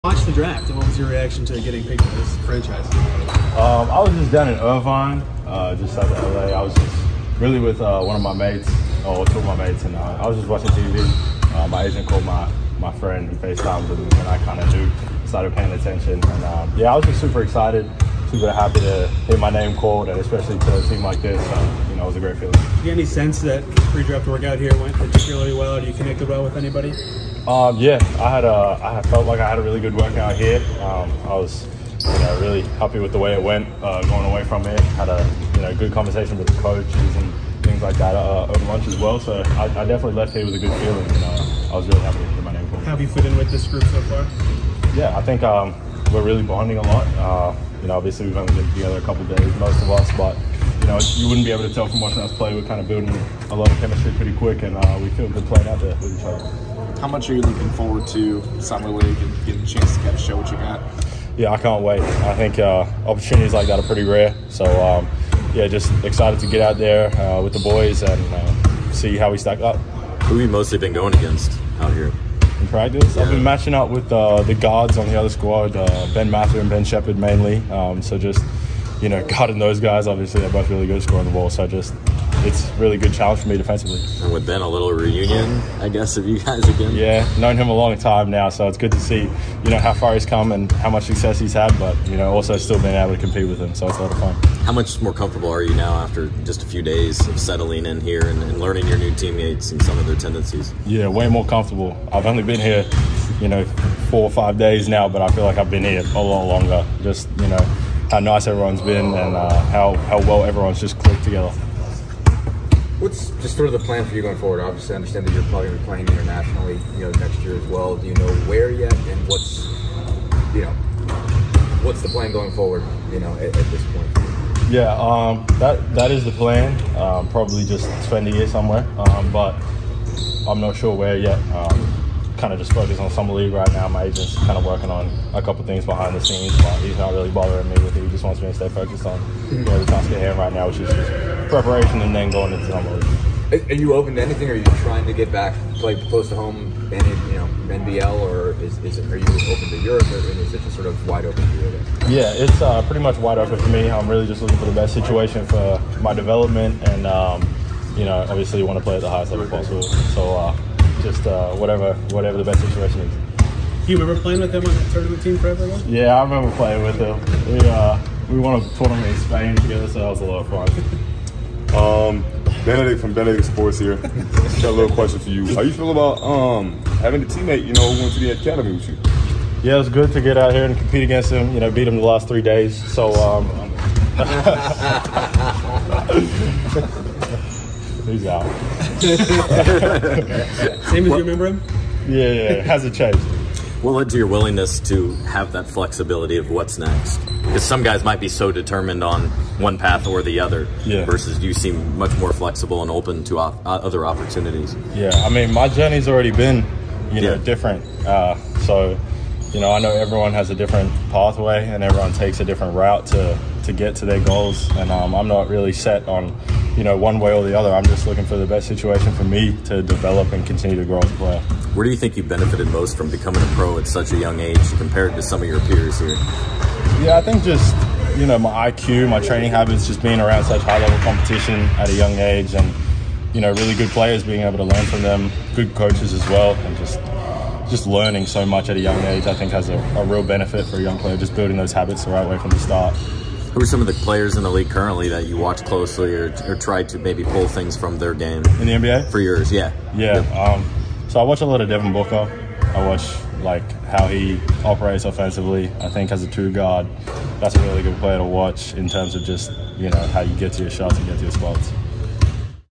Mojave King, Pacers' pick at No. 47, is unsure where he's playing next year
Mojave King took an unusual path to the NBA, going pro as a teenager and then playing for G League Ignite. That path will continue next season away from the NBA, though he's unsure where just yet. His attention is on Las Vegas Summer League and making a lasting impression.

He also spent time playing in the NBA Academy like Bennedict Mathurun, who makes a cameo.
Scott Agness runs FieldhouseFiles.com covering the Pacers.